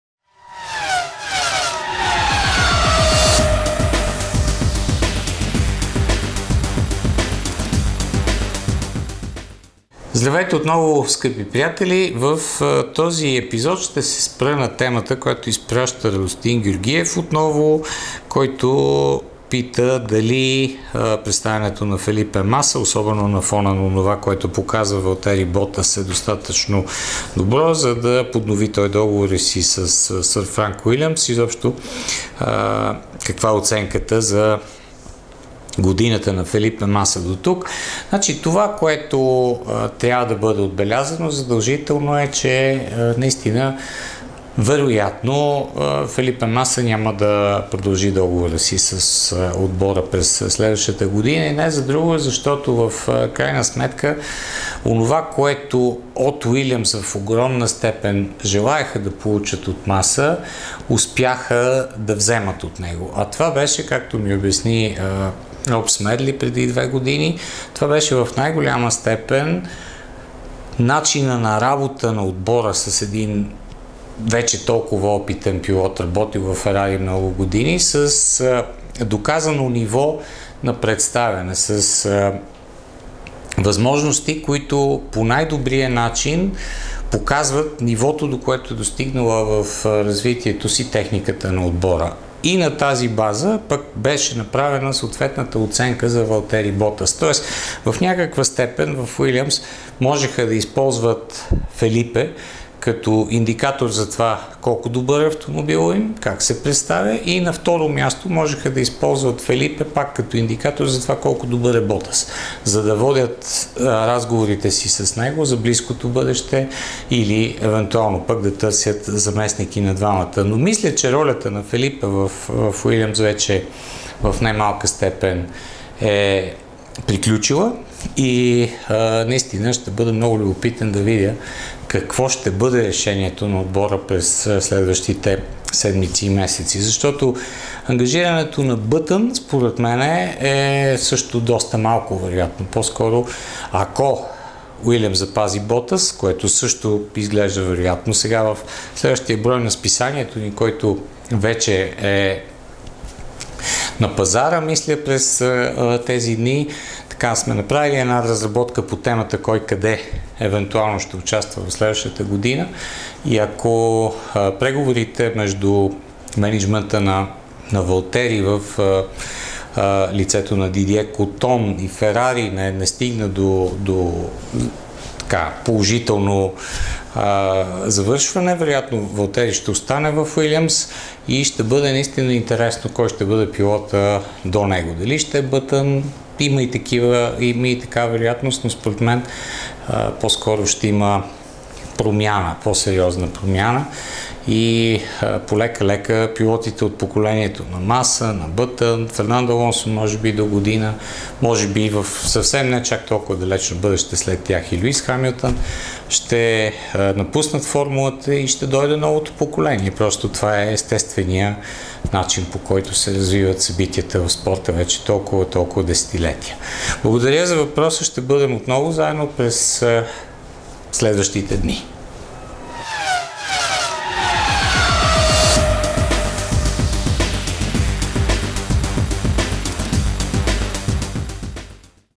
Представянето на Фелипе Маса и какво е неговото бъдеще
Радостин Георгиев ни пише:

Темата, която бих искал да засегна е свързана с представянето на Фелипе Маса до момента през сезона.
Личното ми мнение е, че при този пилот е налице сериозен спад в представянето, което едва ли се дължи само на изоставането на тима му "Williams"  спрямо директните съперници. Валтери Ботас разполага със същата техника, но постига далеч по-добри резултати. На какво се дължи слабата форма на Маса дотук през годината и смятате ли, че той ще поднови договора си с тима на Сър Франк и за догодина или ще бъде предпочетен пилот от ранга на Бътън?